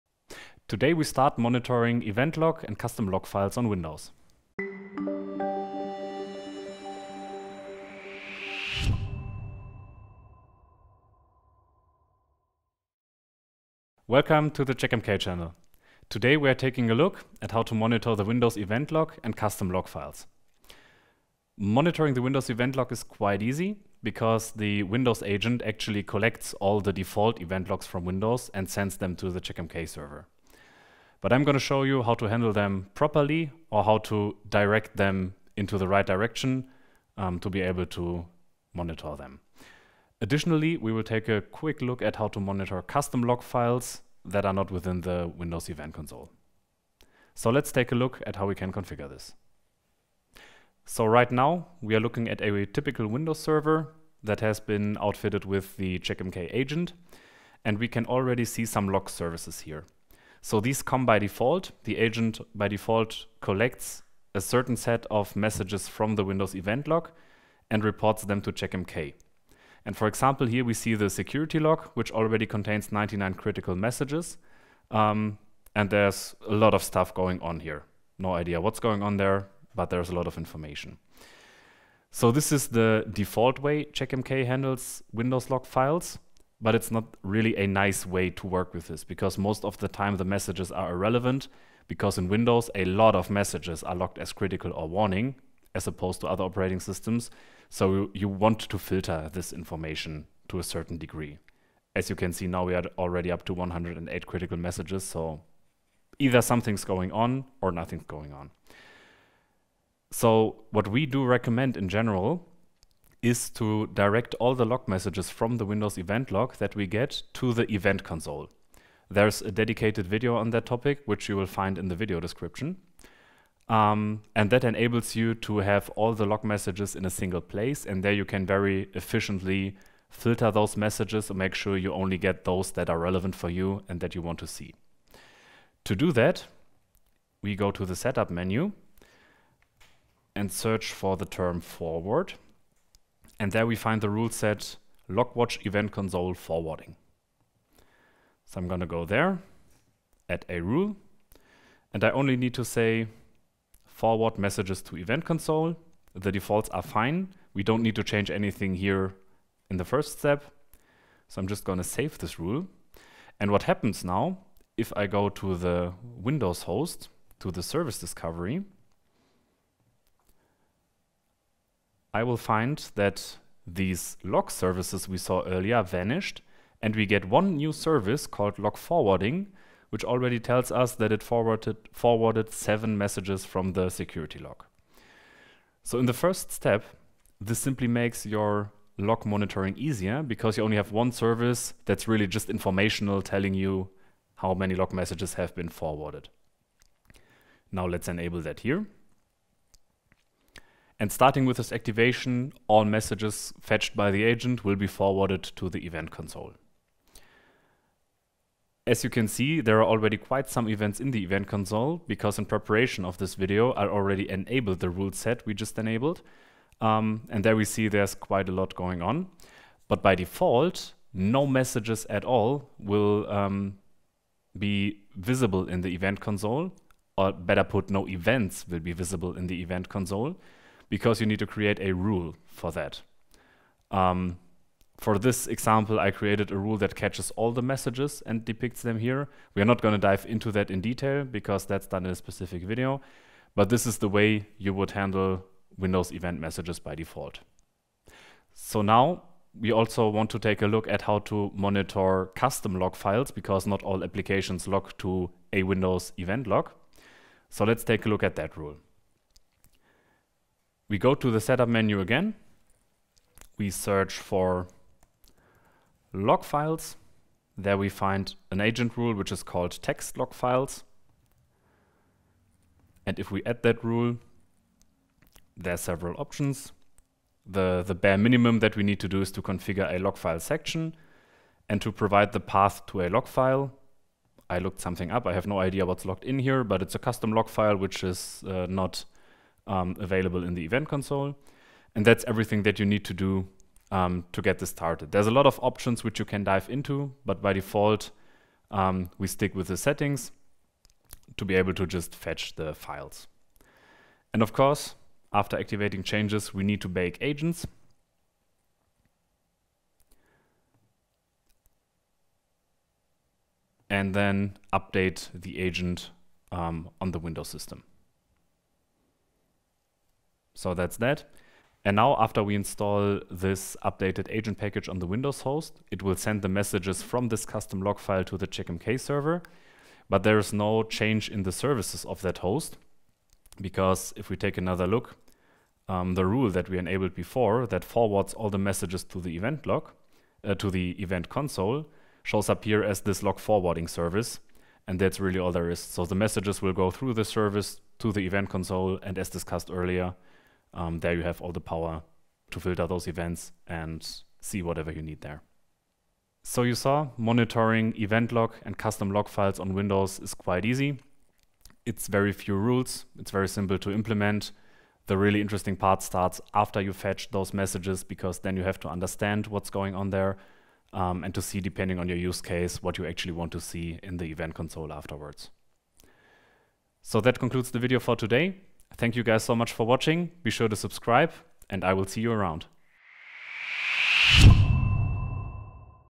Monitoring Windows Event Log and Custom Log Files with Checkmk
In this episode, Robin demonstrates how to monitor windows event logs and custom log files. The Windows agent collects the event logs, but monitoring custom log files is done by creating specific rules. Watch this video to learn more.

00:00 Introduction
00:51 Configuring Logwatch event console forwarding
04:08 Monitoring custom log files
05:55 Conclusion